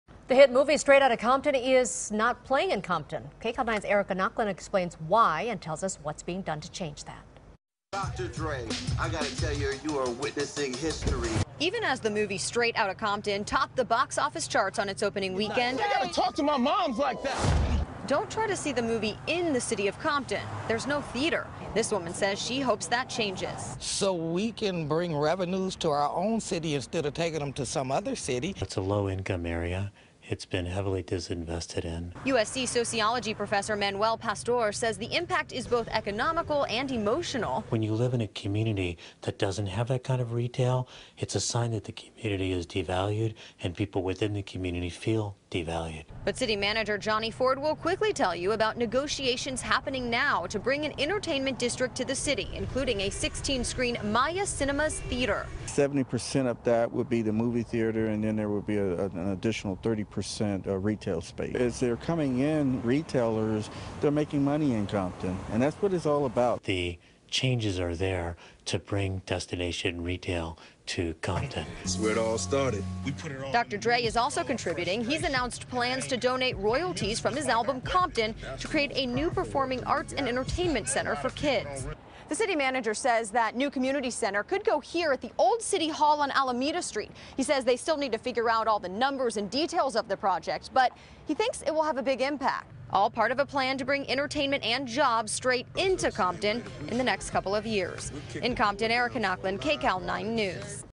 'Straight Outta Compton' Not Showing In Compton Due To Lack Of Theater
The hit film is not playing in its home city, which lacks a theater. Erica Nochlin reports.